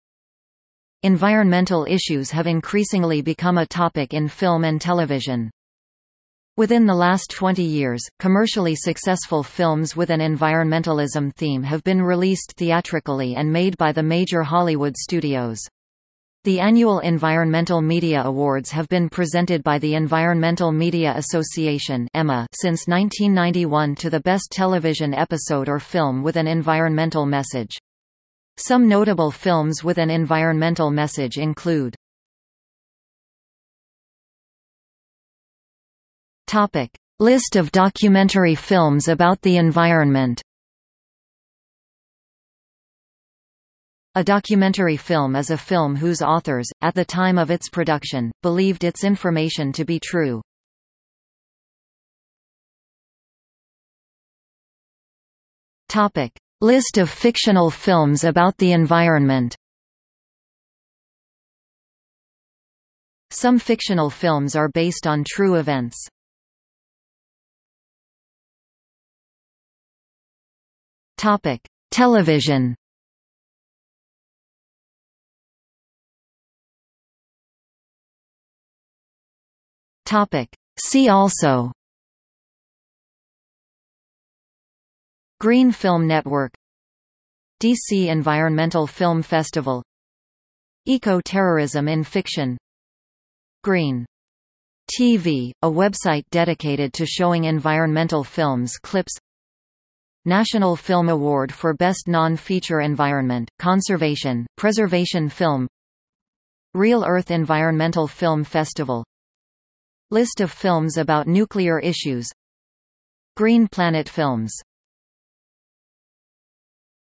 List of environmental films | Wikipedia audio article
00:00:32 1 List of documentary films about the environment
00:00:52 2 List of fictional films about the environment
00:01:07 3 Television
00:01:16 4 See also

- Socrates

SUMMARY
Environmental issues have increasingly become a topic in film and television.
Within the last twenty years, commercially successful films with an environmentalism theme have been released theatrically and made by the major Hollywood studios. The Annual Environmental Media Awards have been presented by the Environmental Media Association (EMA) since 1991 to the best television episode or film with an environmental message.Some notable films with an environmental message include: